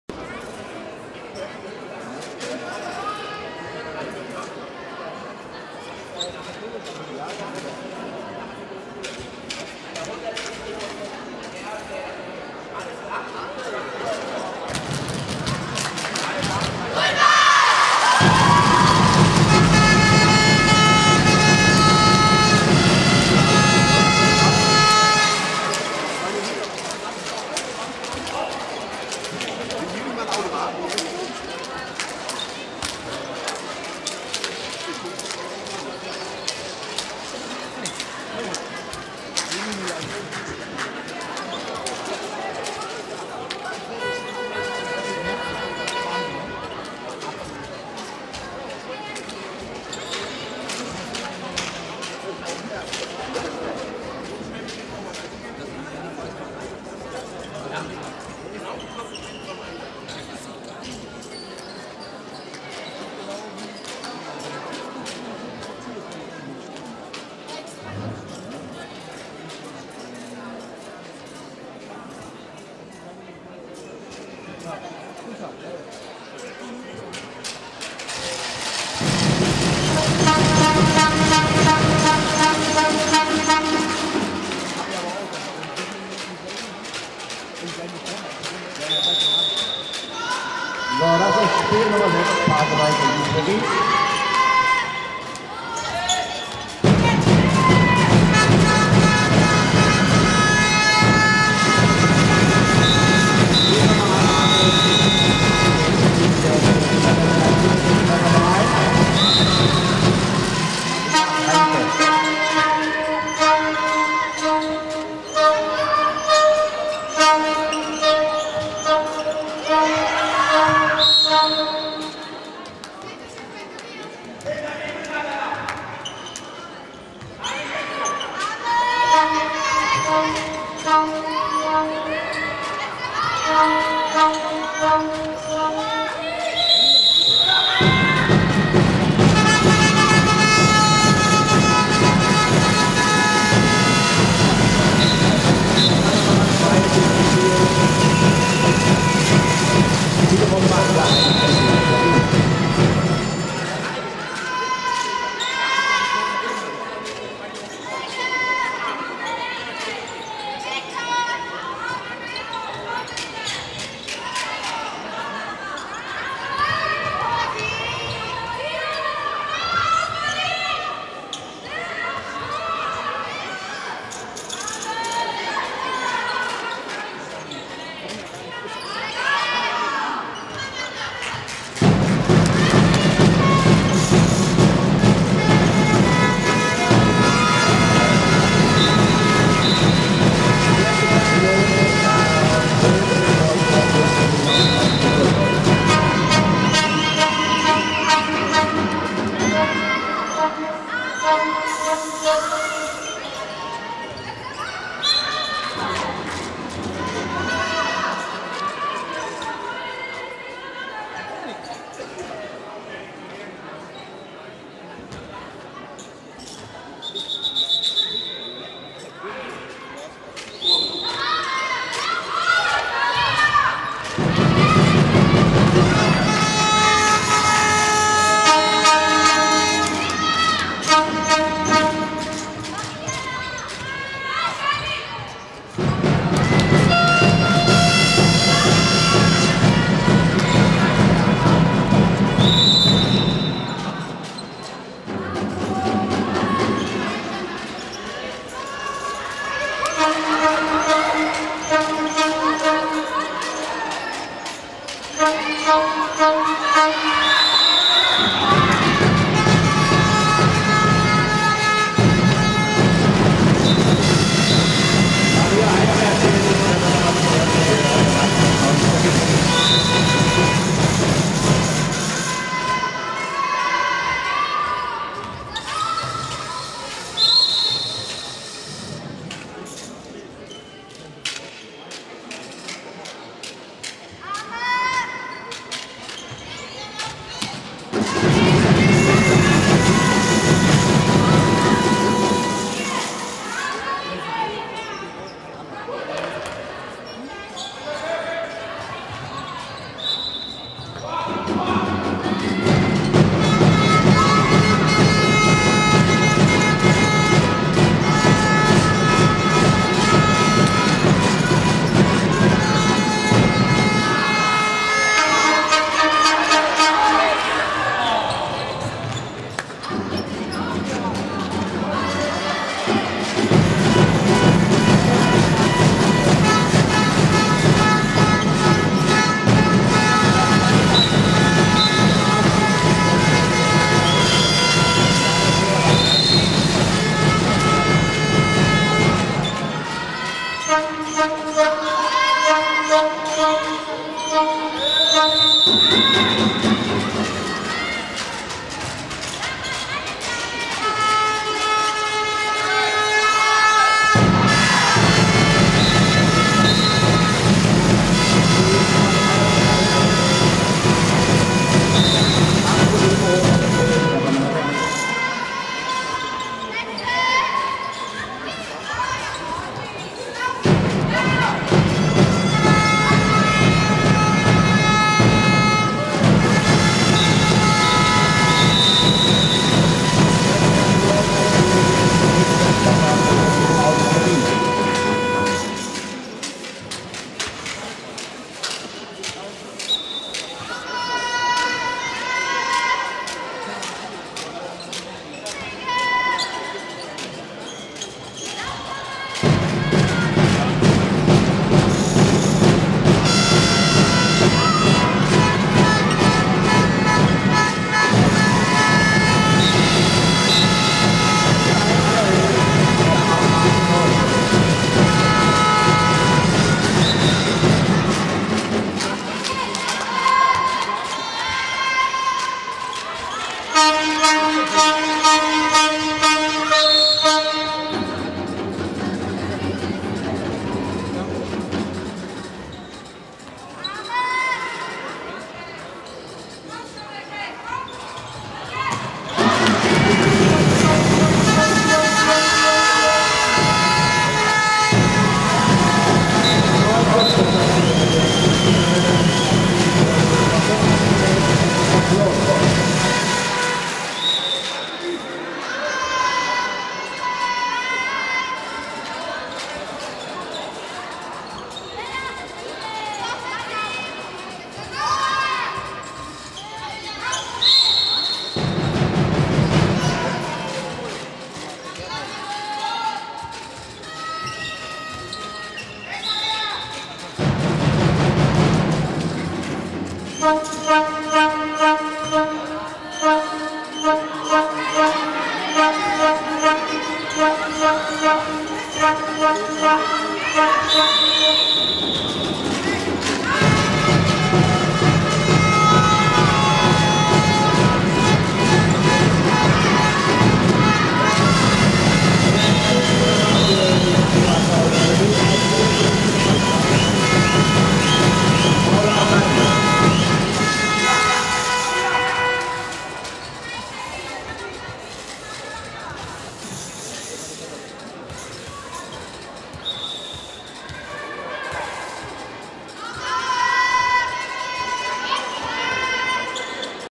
Handball Spielunion wJD Endrunde Landesmeisterschaft 16. Mai 2010
Handball Spielunion wJD Endrunde Landesmeisterschaft 16. Mai 2010 Pasewalker HV vs. SV Grün Weiss Schwerin (4:14)